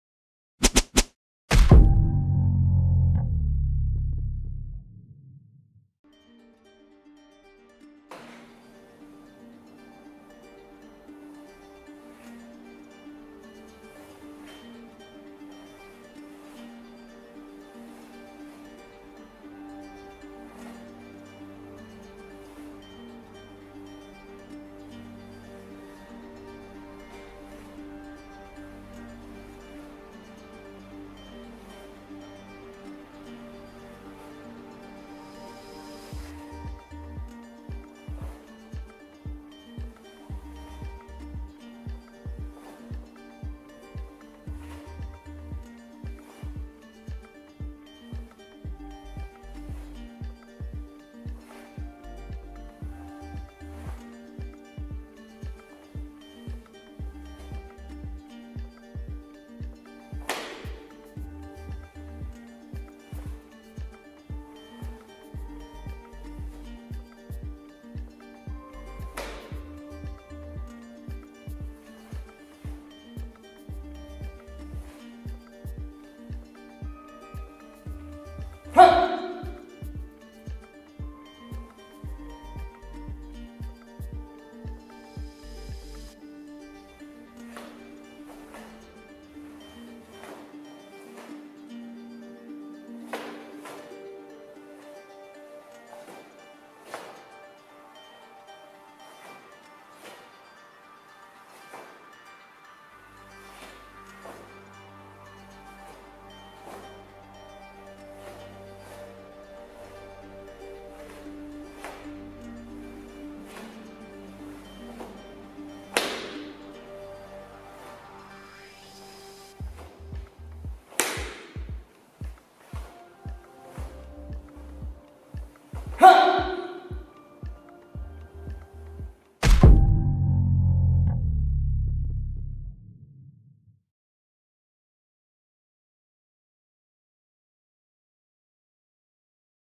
Top secret about Taekwondo Poomsae 5 Jang with hand skills
| 태권도 태극품새 5장 손동작

Hello everyone

Team_tearo is here!

Our new content is poomsae hand techniques without kicking and stances.

If you improve about hand techniques in Taegeuk poomsae no.5

Hand's skills are the most important part in poomsae

Let's try this first! then build up your improvements

It is the mirror mode,

just try to follow those motions what you see,